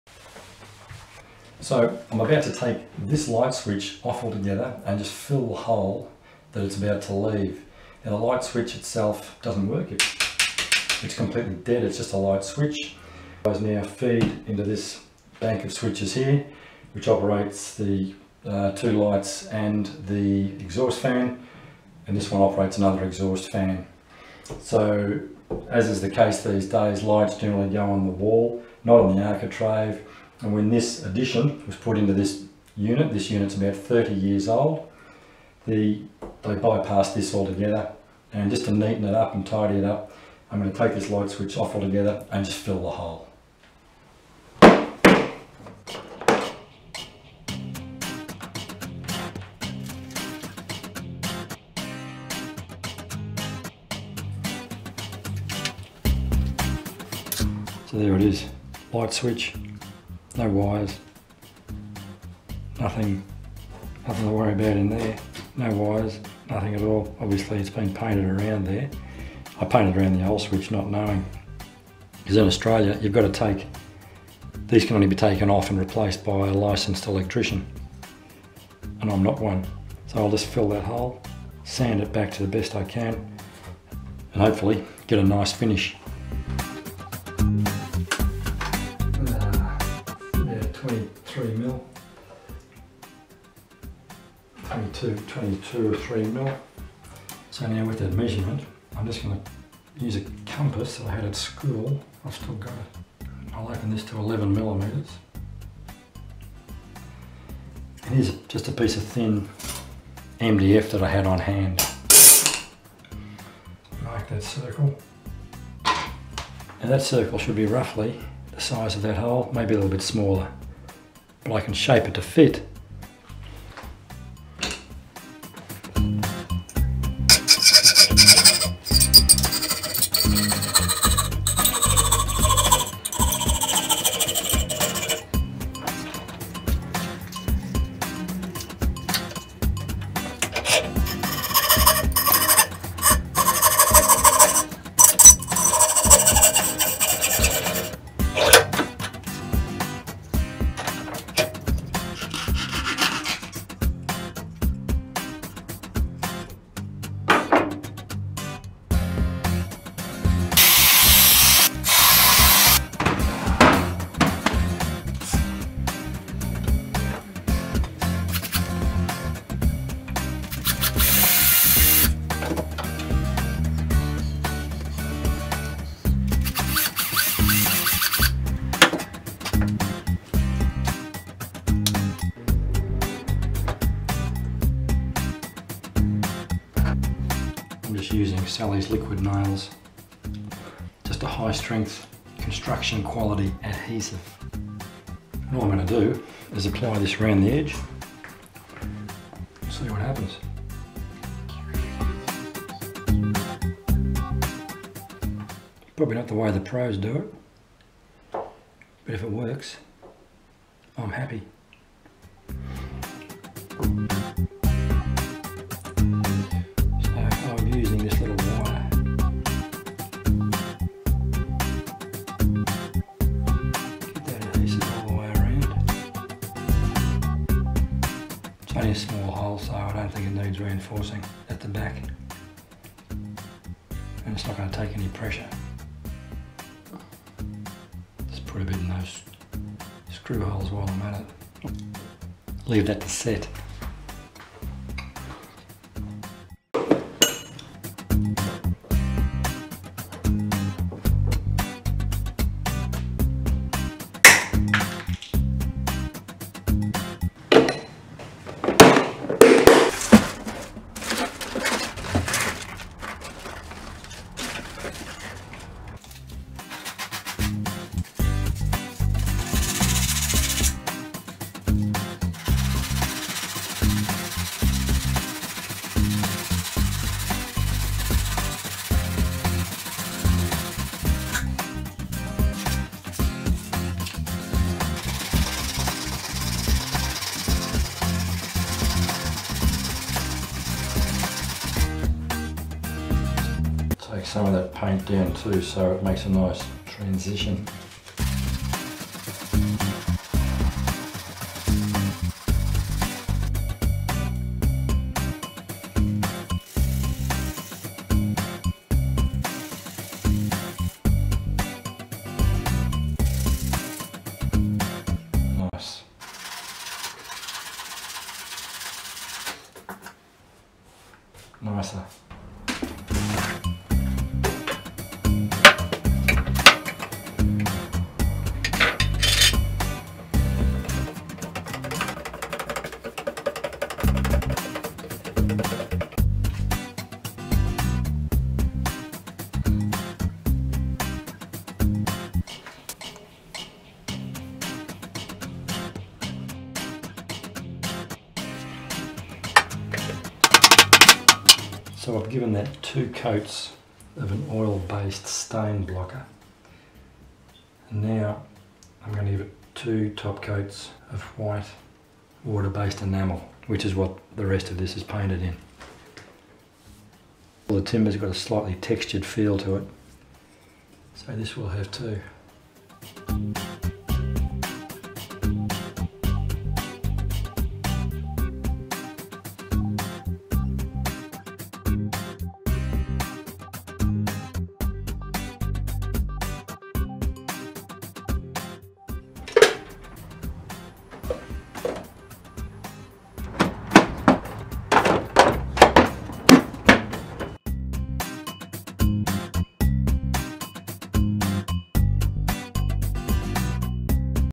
How To Repair a Small Hole In Your Door Frame.
This video shows a very easy repair to a hole in an architrave/door frame left behind when I took out an unconnected light switch.
My goal was simply to tidy up the architrave and leave a smooth finish. The area is not weight bearing so a simple cosmetic job was sufficient.
I'm not a tradesman and it's surprising what can be done when you just have a go (within reason of course!).
Also, I had all the materials on hand so it didn't cost anything.
They wouldn't have cost much anyway as you could just poke a hole in the MDF (with an awl or similar) to put the wire through and the construction grade adhesive is only a few dollars.
It's nice to do it yourself if you can.

Cheers, Mark.👍

Edited on Windows Movie Maker